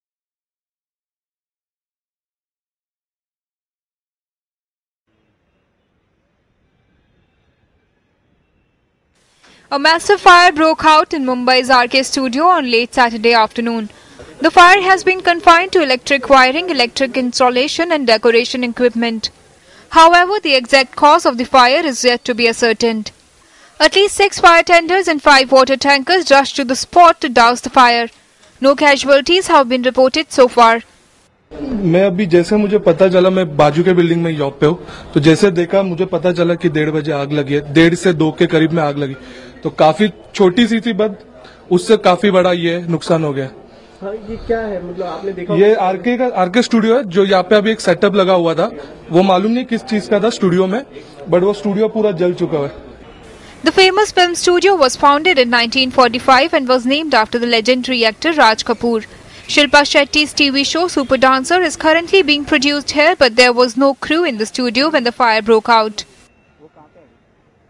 Major blaze at R.K. Studios guts reality show set
A major fire broke out in the iconic R.K. Films and Studios in Chembur here on Saturday afternoon, BrihanMumbai Municipal Corporation (BMC) Disaster Control officials said. At least 11 fire tenders and ambulances rushed to the sprawling studio in north-east Mumbai and brought the conflagration under control after over an hour.